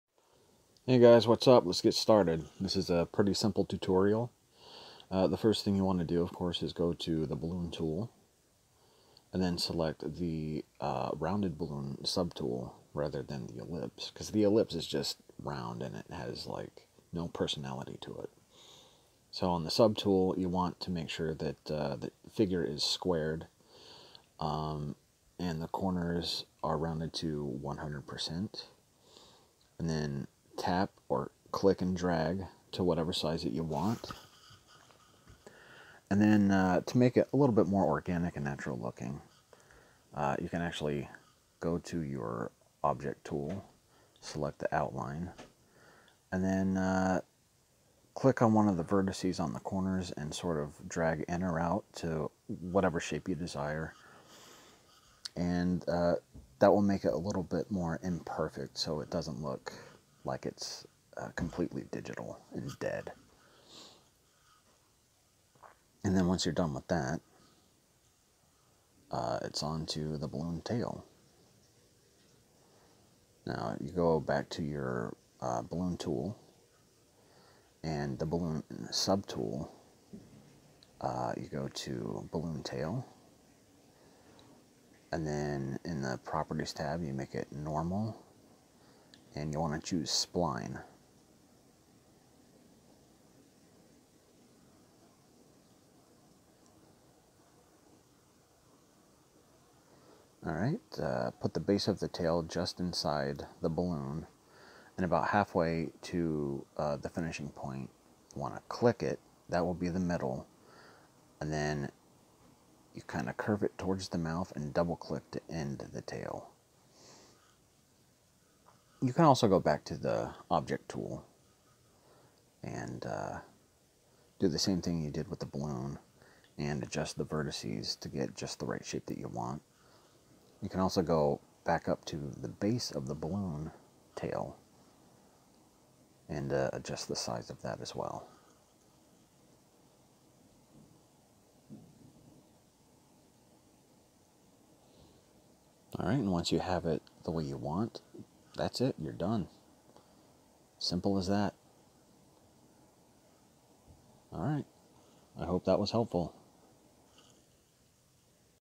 Creating a Word Balloon in Clip Studio Paint (Using Samsung Galaxy Tab S6 Lite)
A quick guide for creating a normal word balloon (for comic books) in CSP on an Android tablet.